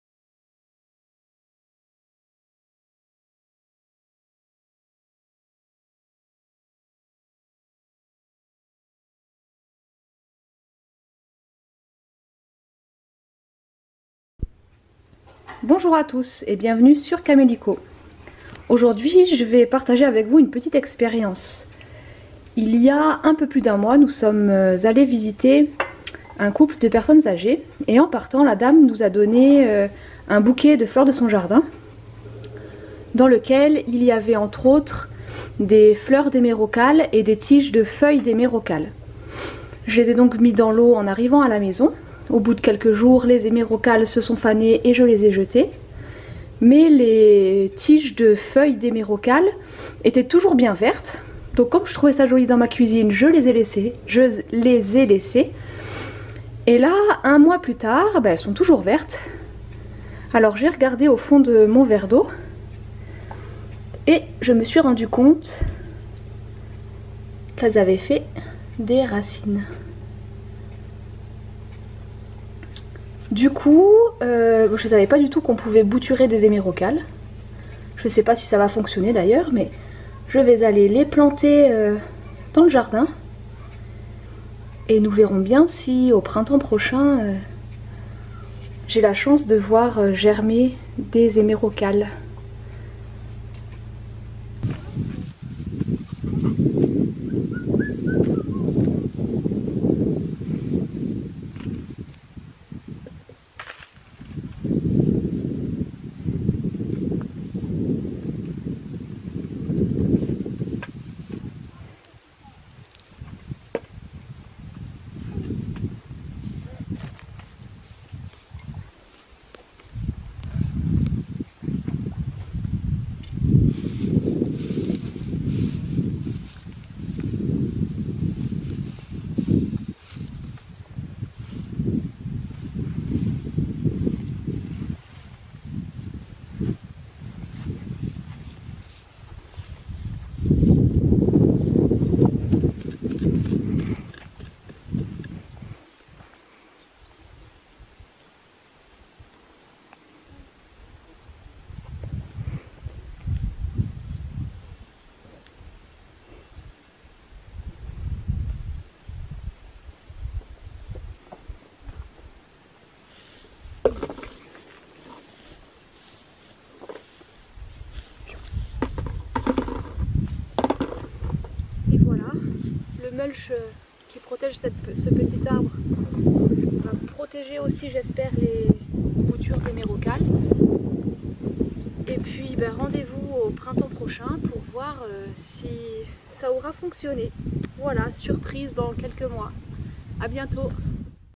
Fleurs #1 - Bouturer les hémérocalles
Les fleurs, j'adore ça... Les insectes aussi d'ailleurs, ce qui fait que les plantes potagères (et les arbres) apprécient aussi leur compagnie.
J'aimerais vraiment que notre lieu de vie soit tout fleuri ! Pour cela, j'avais préparé une zone spéciale au-dessus du potager, mais je veux aussi installer des fleurs un peu partout sur le terrain... Je commence par une expérience : repiquer des tiges de feuilles d'hémérocalles qui ont fait des racines dans l'eau...

Merci à vous, et à bientôt pour de nouvelles vidéos !